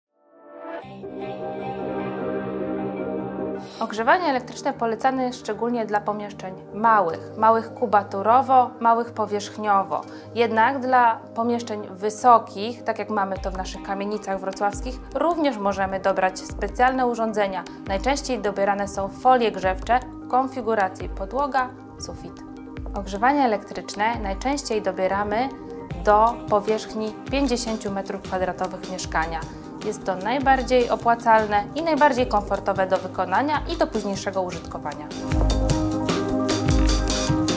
Zmień piec: Ekspert radzi - Dla jakich pomieszczeń zalecane jest ogrzewanie elektryczne?
– Najłatwiej z ogrzewania elektrycznego korzysta się w mieszkaniach do powierzchni 50 mkw. Jednak w przypadku tych większych i wyższych pomieszczeń także można dobrać odpowiednie źródło energii. W pokojach stosujemy najczęściej urządzenie magazynujące o odpowiedniej mocy, które stopniowo uwalnia ciepło, np. piec akumulacyjny. W kuchni czy w łazience wystarczy mniejsza instalacja, która będzie ogrzewała pomieszczenie tylko wtedy, gdy tego chcemy. To najbardziej opłacalne oraz komfortowe do wykonania i użytkowania rozwiązania – mówi Zofia Grojec, kierownik Zespołu Technicznej Obsługi Zasobów w spółce Wrocławskie Mieszkania.

Obserwuj profil „Zmień piec” na Facebooku i bądź na bieżąco ze wszystkimi informacjami.